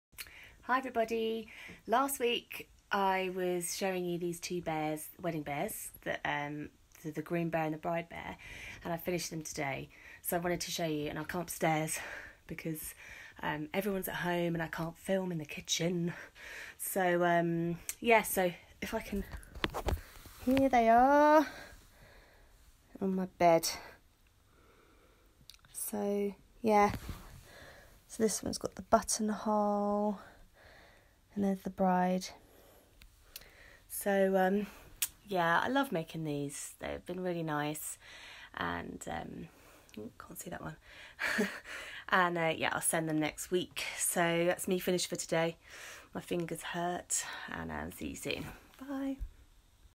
Bowman Bears wedding dress bear and groom bear
Hi guys, I have made two lovely wedding keepsake bears. The wedding dress bear had been made from two wedding dresses and the Groom bear has been made from the Groom’s shirt and the waistcoat has been made from the wedding dress.